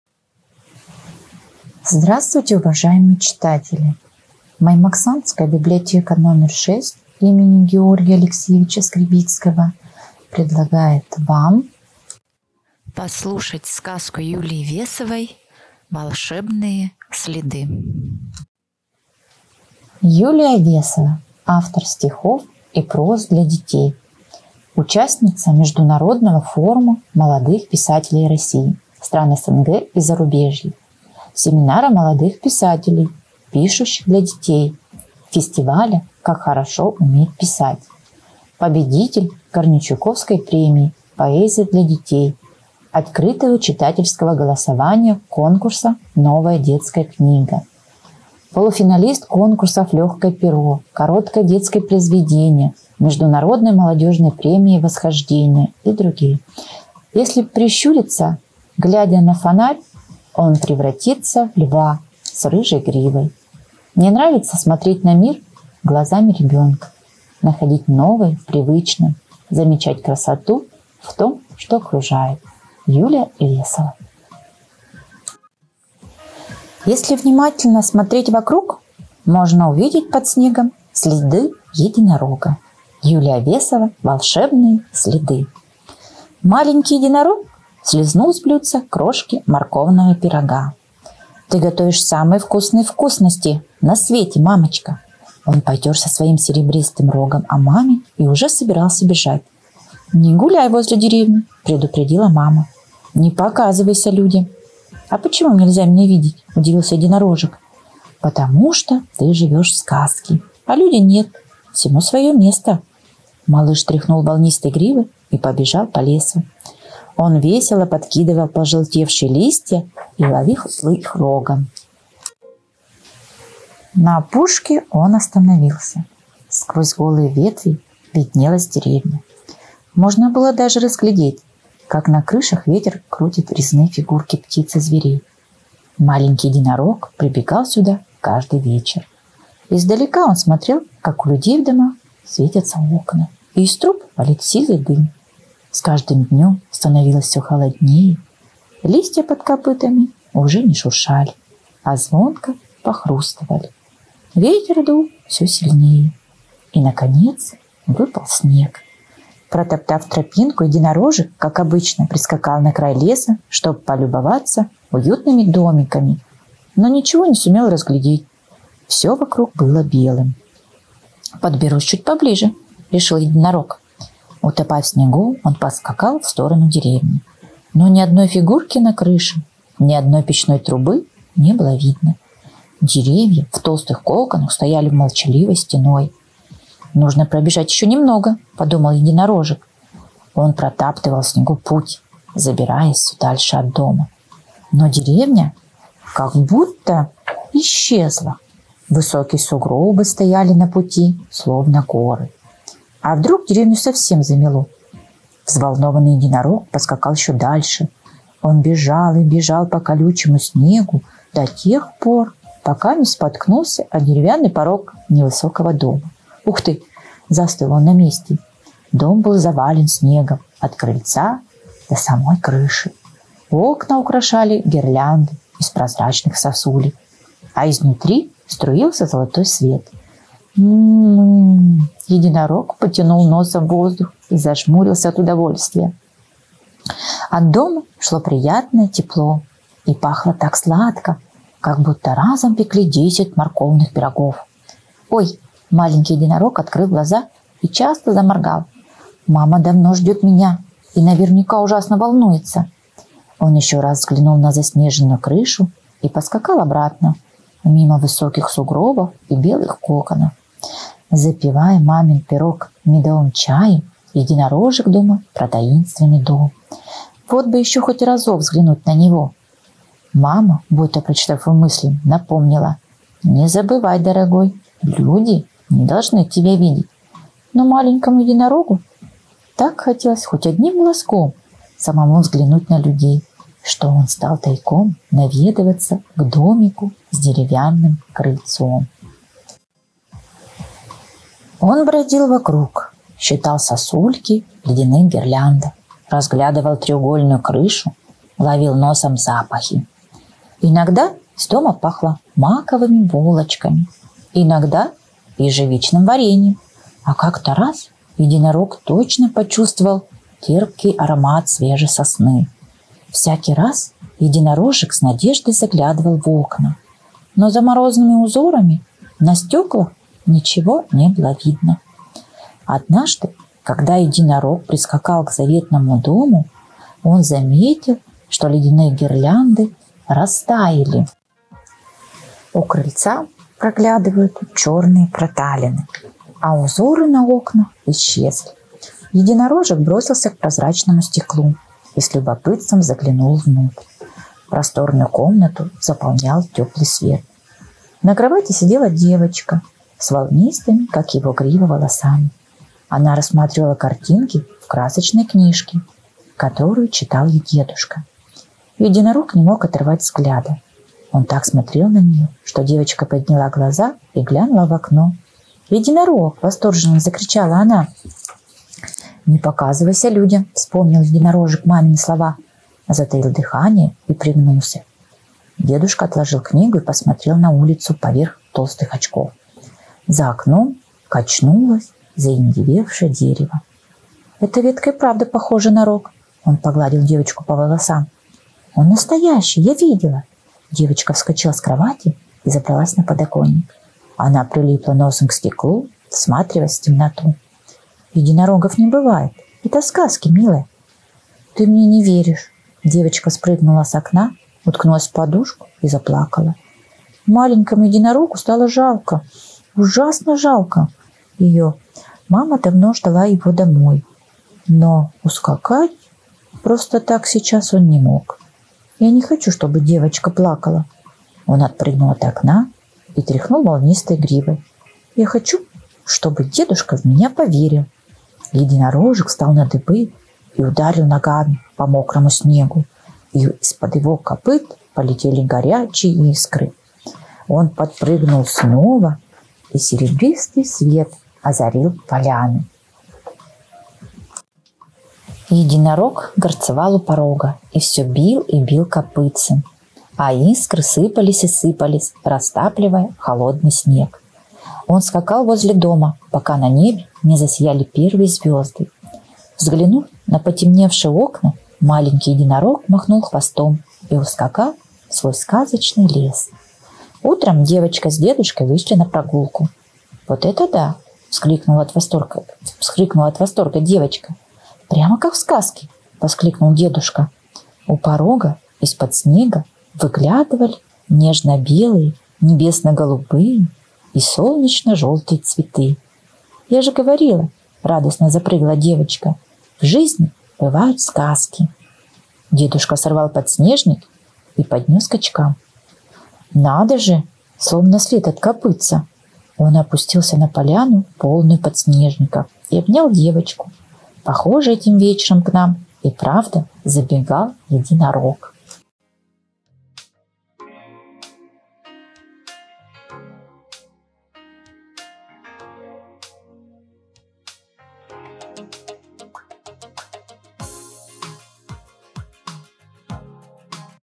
Юлия Весова «Волшебные следы»
В Маймаксанской библиотеке № 6 им. Г. А. Скребицкого предлагают послушать сказку современного автора стихов и прозы для детей.